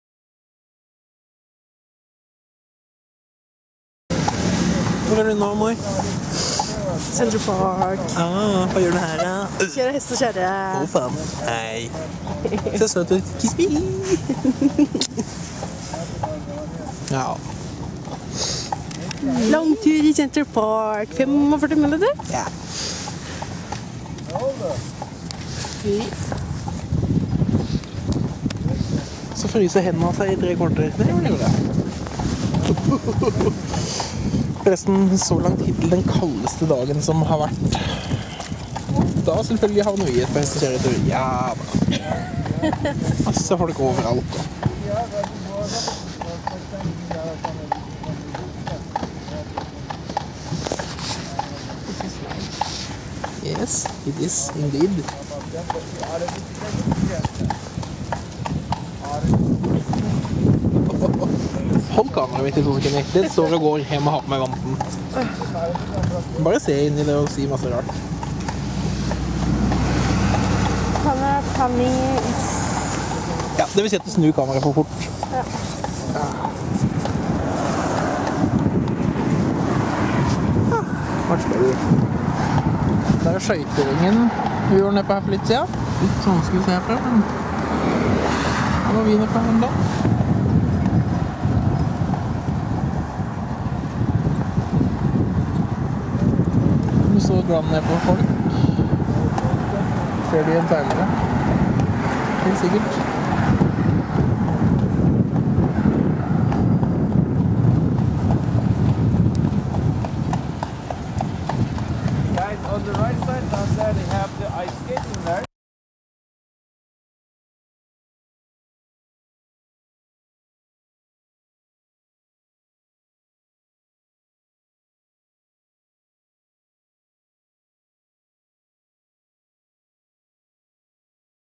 Horse and carriage in Central Park - Panasonic HDC-SD80EC-K
Haveing the long ride in horse and carriage in Central Park. 45 min.
And offcorse we had to do it in the coldest day of our vacation in New York